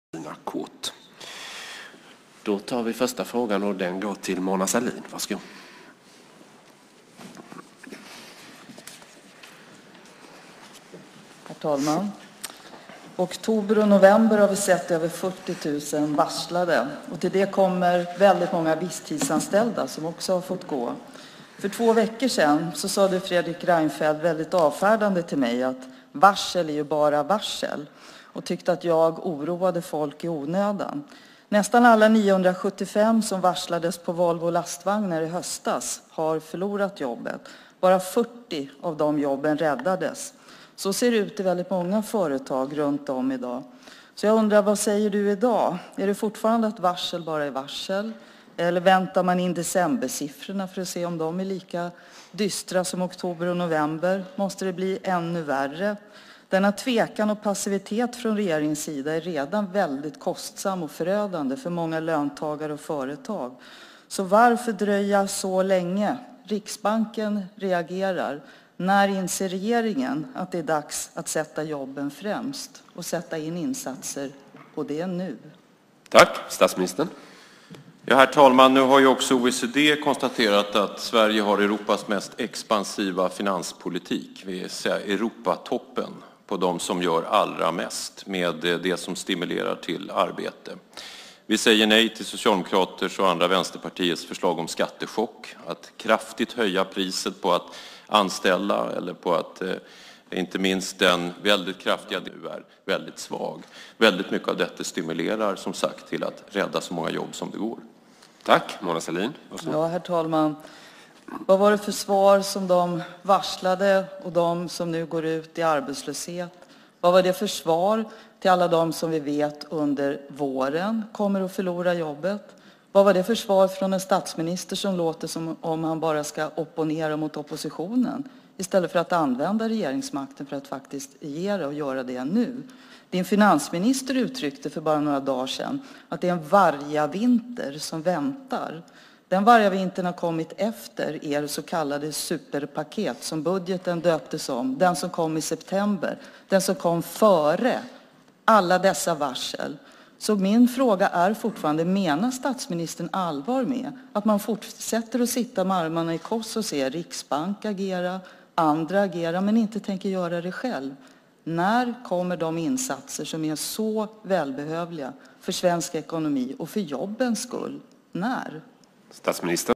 Mona Sahlin i riksdagens frågestund om varsel- och jobbkris
Mona Sahlin ställer frågor om varsel- och jobbkrisen till Fredrik Reinfeldt.